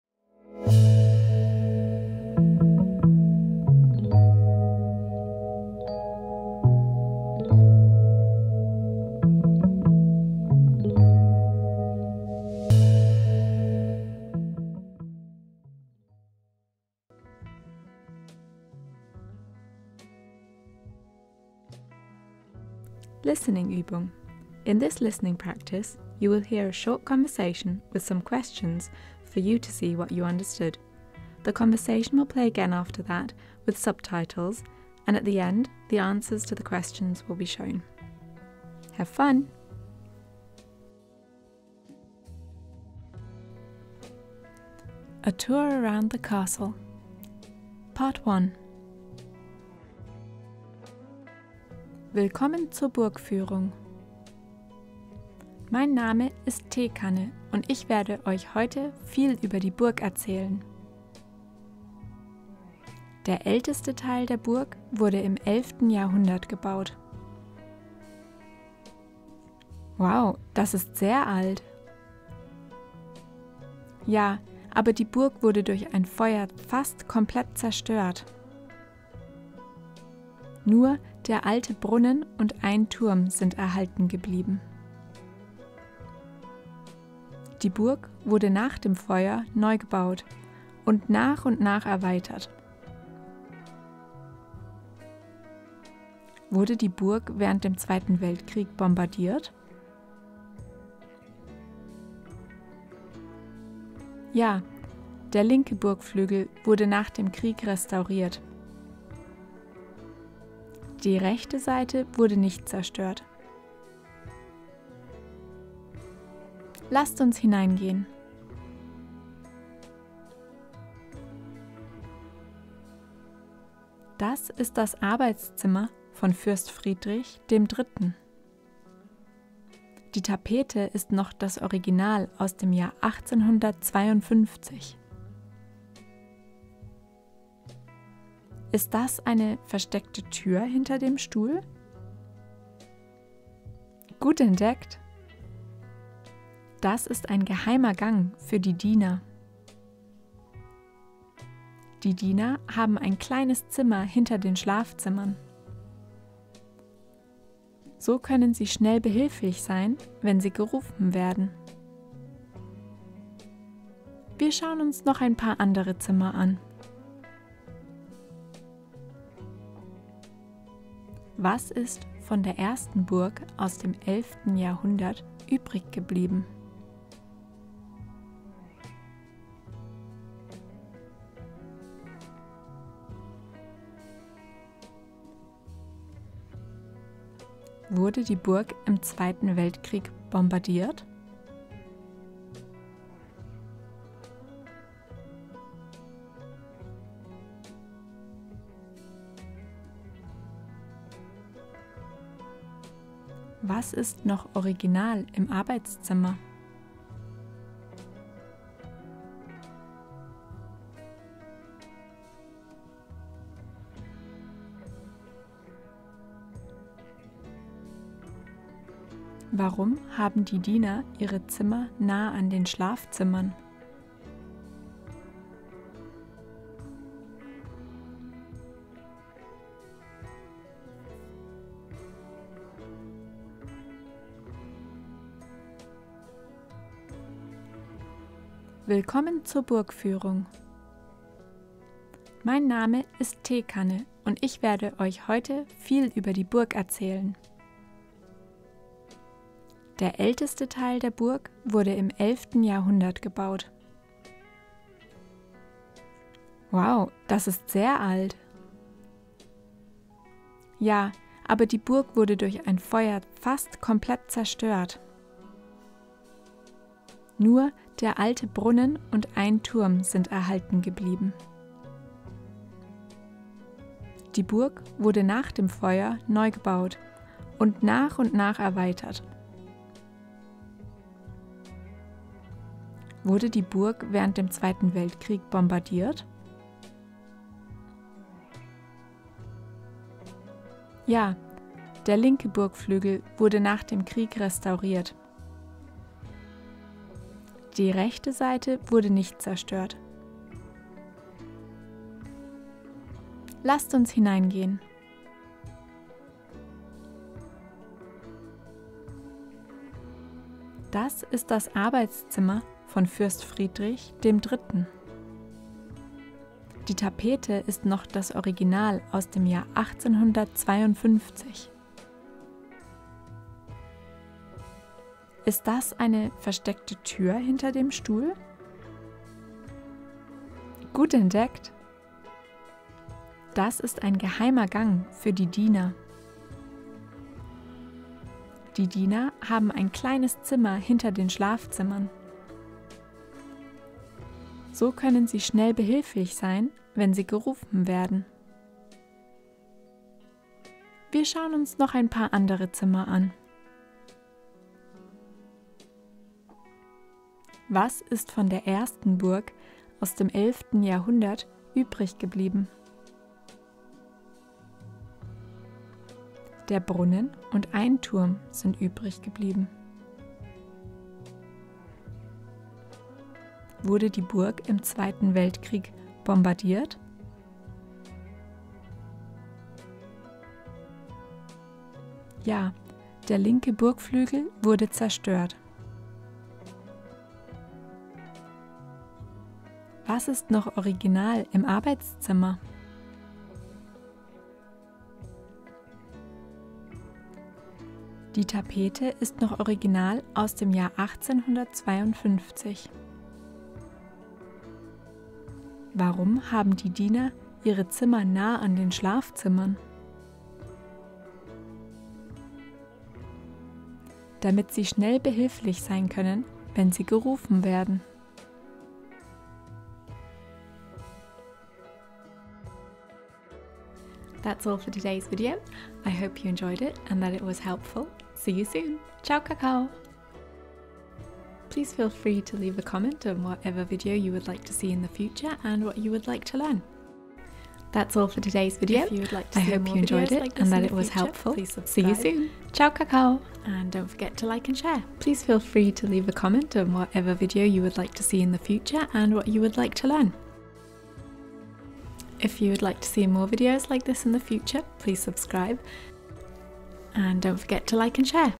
Difficult Realistic Conversation| A Tour Around the Castle Part 1 | Learn German
Hi everyone!
Let me introduce you to Pretzel and Tea cup - these two will guide you on your adventure to learn some German!
In this video you will hear a listening comprehension modelling a tour guide situation.
Let me know what kind of videos you would like to see in the future!
Also if you like, let me know where you are from in the comments, as I find that super interesting!

Ciao Kakao!